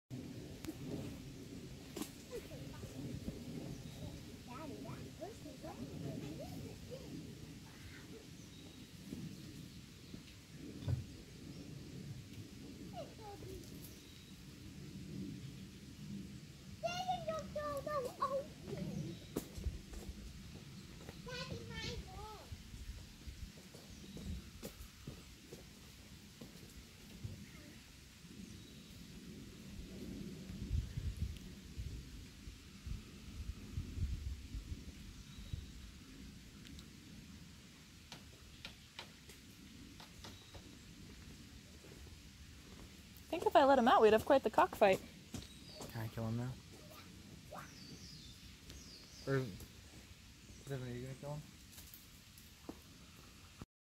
Head Rooster Challenges Intruder Cockerel
A neighbor gave us an extra cockerel to eat. While we were prepping, our resident rooster discovered him and was a bit threatened.